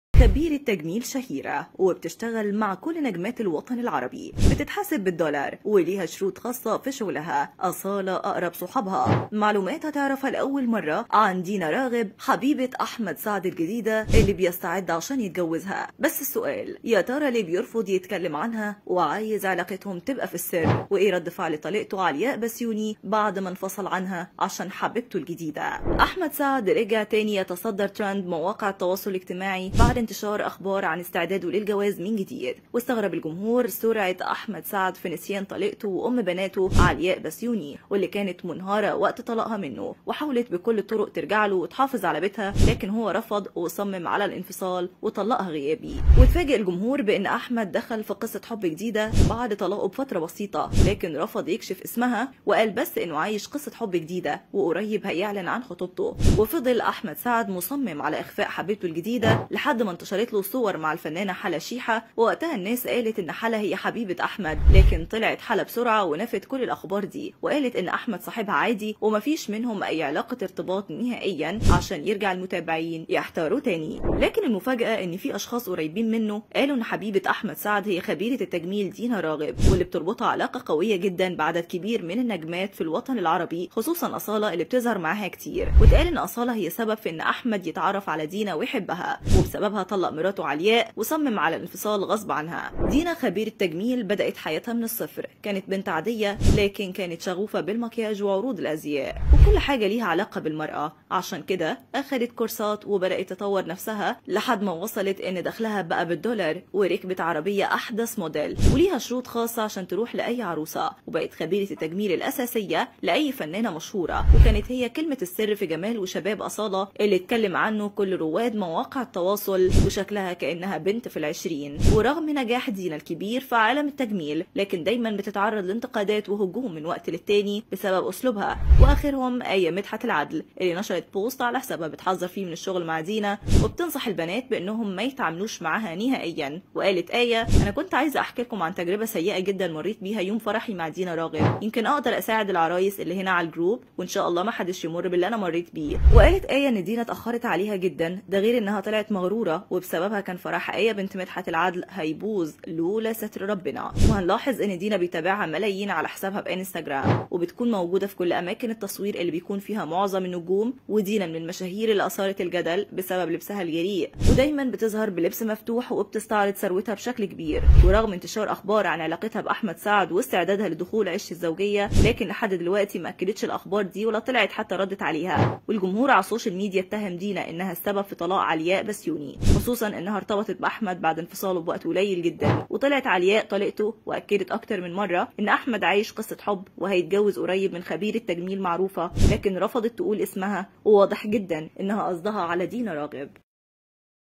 خبيرة تجميل شهيرة وكانت هتبوظ فرح بنت فنان شهير.. تعرف على حبيبة أحمد سعد الجديدة
حلقة نستعرض خلالها تفاصيل ومعلومات عن حبيبة أحمد سعد الجديدة وإزاي بقت خبيرة تجميل شهيرة بتلعب بالفلوس لعب وبتشتغل مع كل نجمات الوطن العربي بتتحاسب بالدولار وليها شروط خاصة في شغلها أصالة أقرب صحابها وليه بيرفض يتكلم عنها وعايز علاقتهم تبقى في السر وإيه رد فعل  طليقته علياء بسيوني بعد ما انفصل عنها وخرب بيتها علشان حبيبتها الجديدة..
تابعنا على منصة كل النجوم